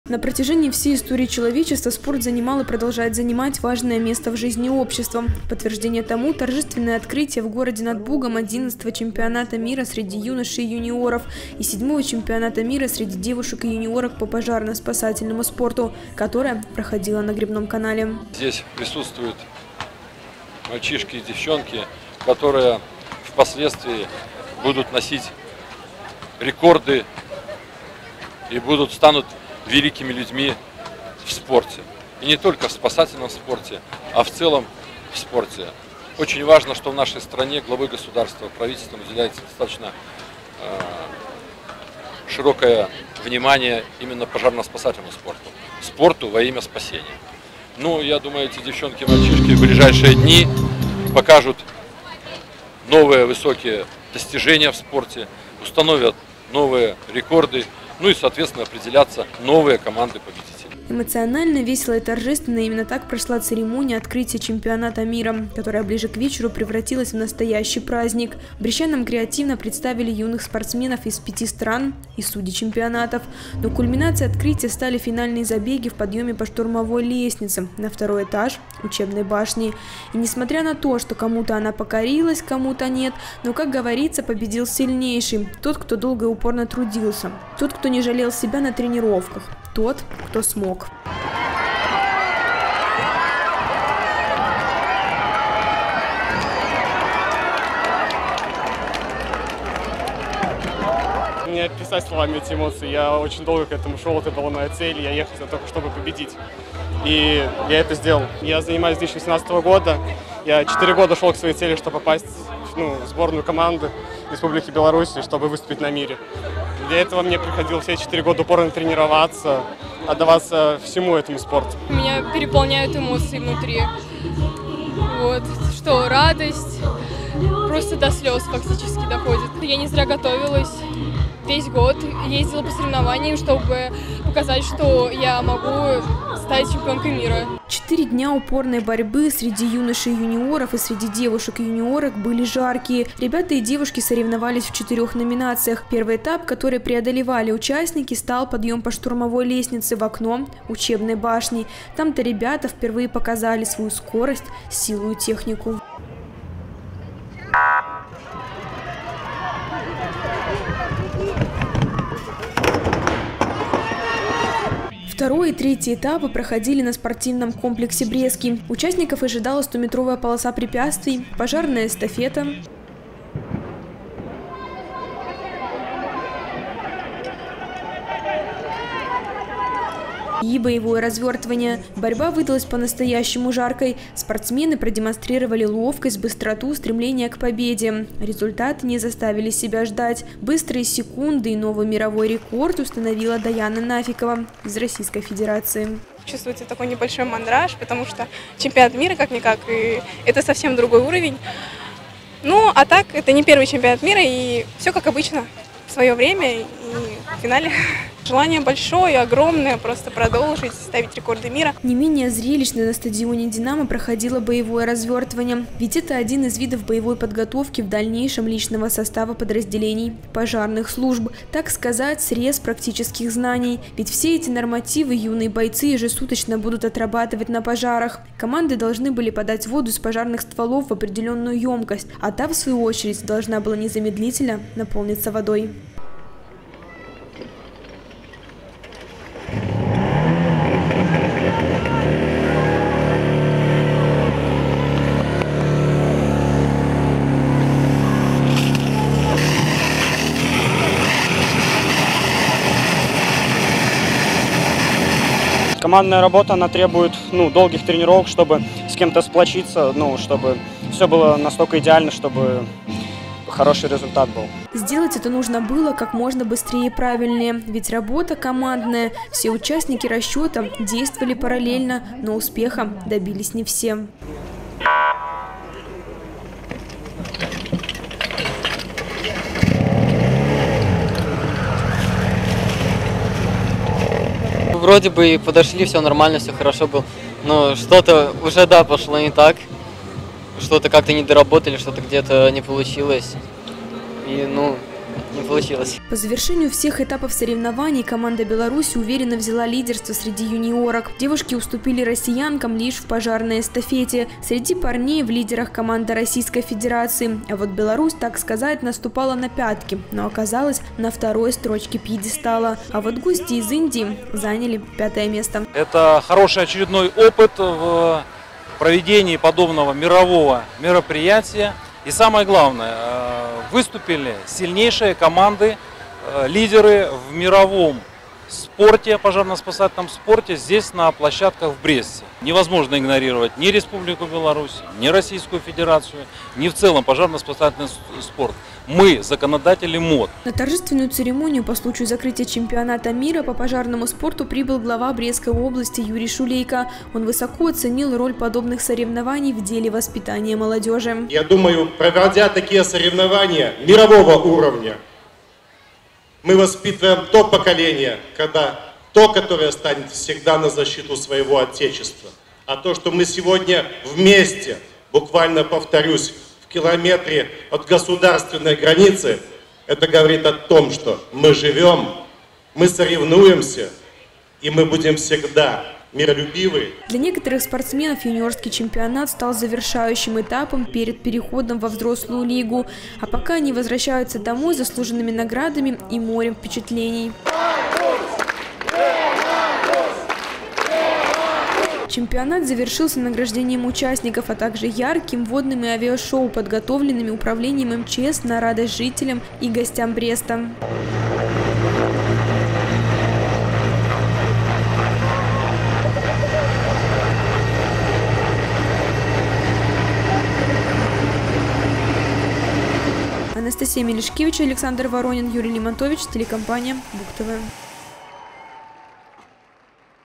2022-08-23 г. Брест. Завершение Чемпионата мира. Новости на Буг-ТВ.
Журналист А. Мелишкевич. Оператор А. Воронин. Э. Бакунович

Чемпионат мира по пожарно-спасательному спорту завершился. Напомним, что проходили соревнования в городе над Бугом с 18 по 22 августа. Они включили в себя два первенства: одиннадцатый чемпионат мира среди юношей и юниоров и седьмой чемпионат мира среди девушек и юниорок. На протяжение четырёх дней 5 стран боролись за звание чемпионов мира. Позитивное настроение, заряд энергии, воля к победе - всё говорило о том, что настроены эти юноши и девушки решительно и борьба действительно нешуточная. Юные-спасатели соревновались в подъеме по штурмовой лестнице, в преодолении 100-метровой полосы препятствий, боевом развертывании и пожарной эстафете 4 по 100 метров. О том, как прошли все 4 дня соревнований и кого по праву можно назвать Чемпионами мира, в сюжете Анастасии Мелишкевич.